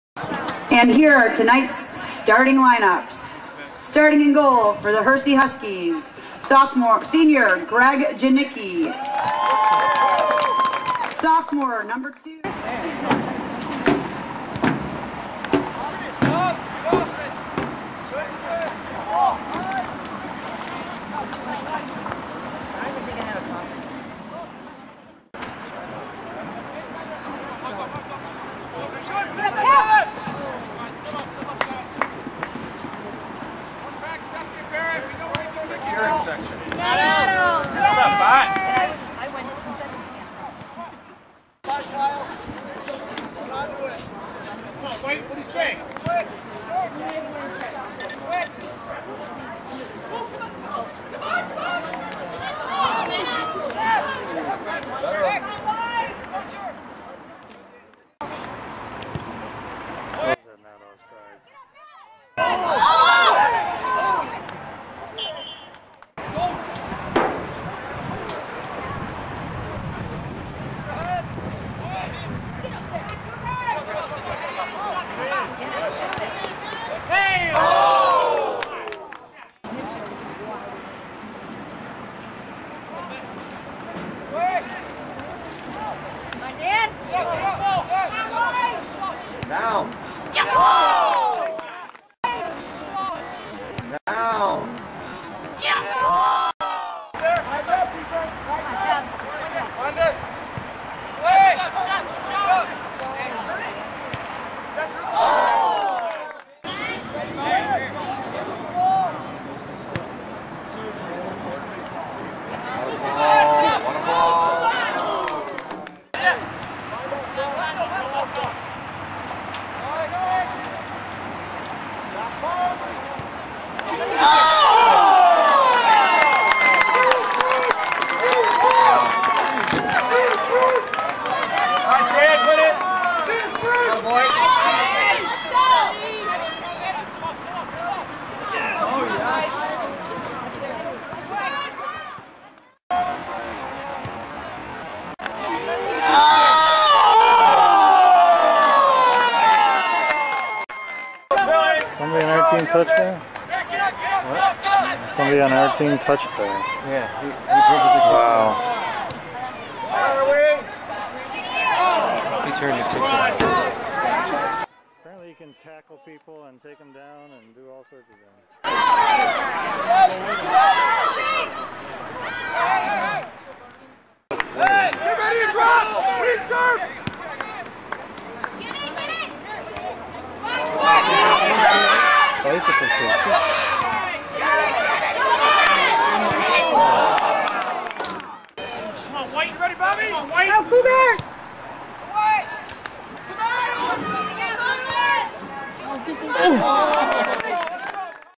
Janicki highlights - 2009 Hersey at Conant
Senior goalie Greg Janicki makes big saves and starts offensive runs with his solid goalie play.